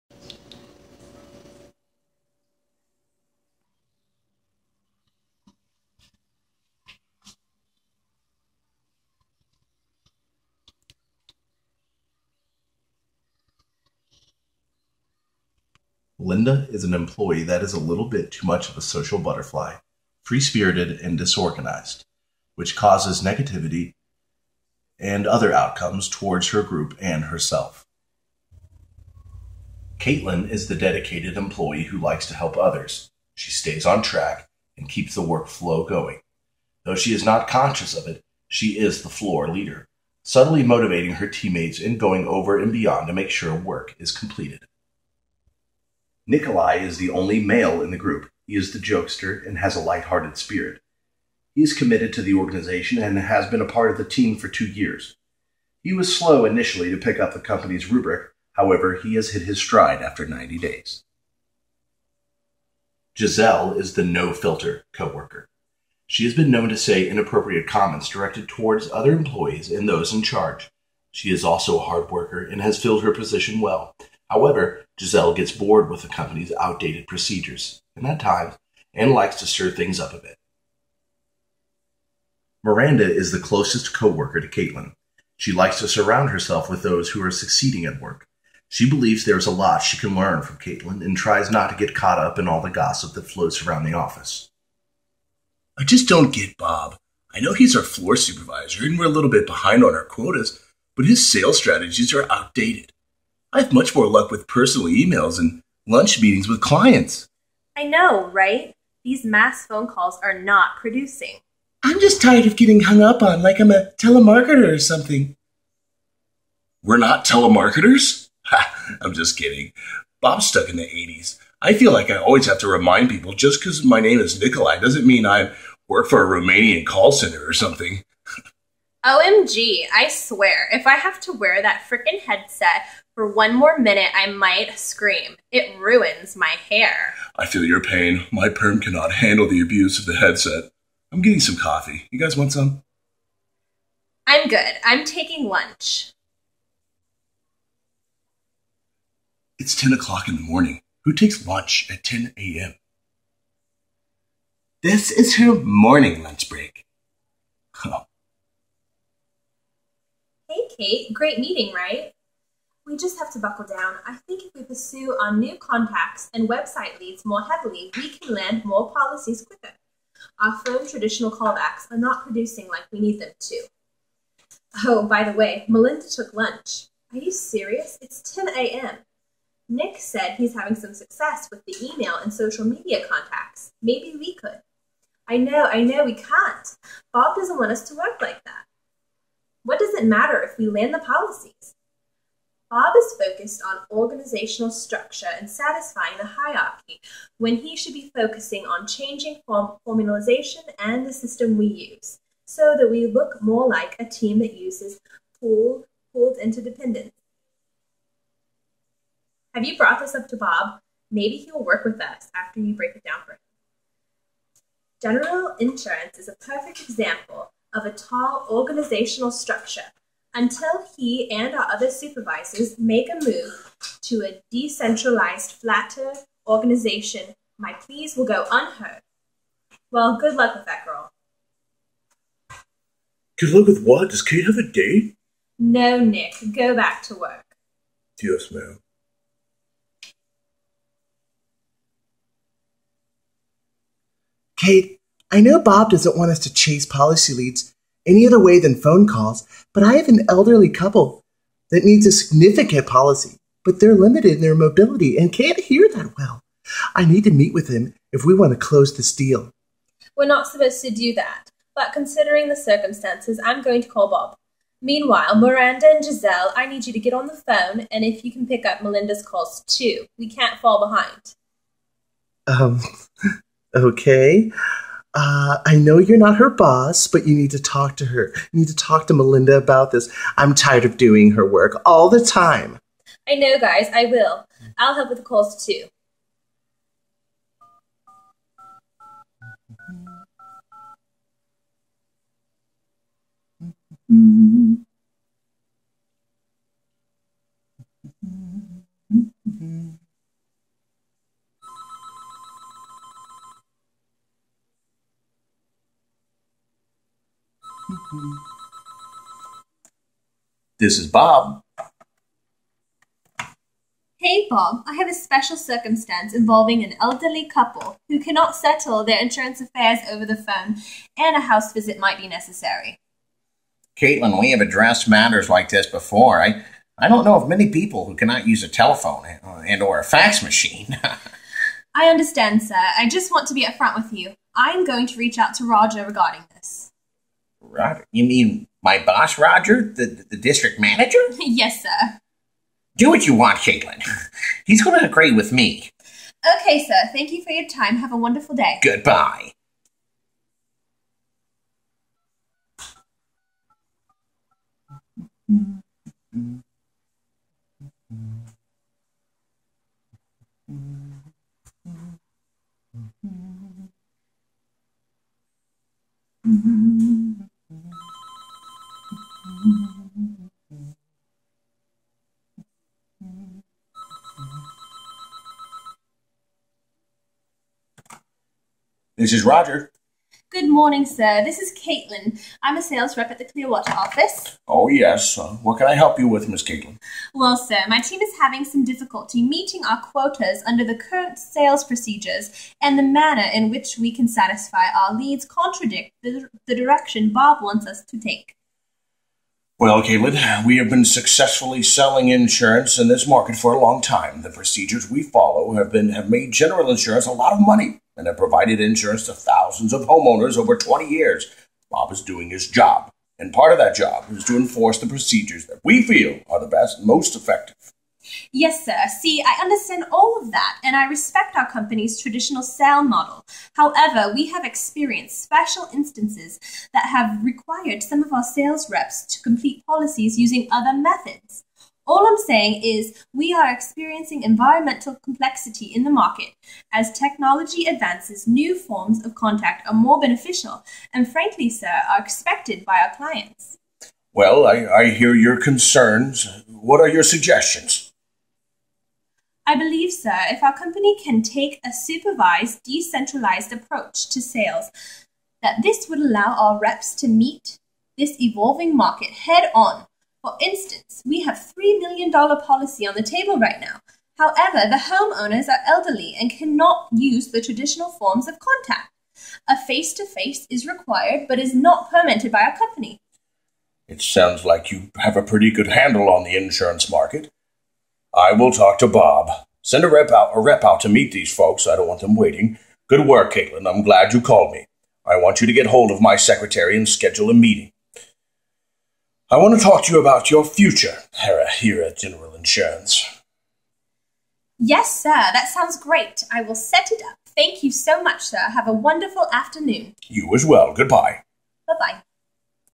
Group Design Challenge #3-MANAGEMENT MAVERICKS
Adam Boggs, Haley Hill, Lauren Seils, Kaitlyn Steakley, Kristy Thor

THEME: the employees are experiencing difficulties dealing with outdated procedures and policies.  Their frustration lies in the centralized work organization with all of the decision making coming from the higher ups.  The group feels they should have more authority on day to day decision making which leads them to ask for added flexibility lower on the hierarchy and shifting to a more decentralized working structure.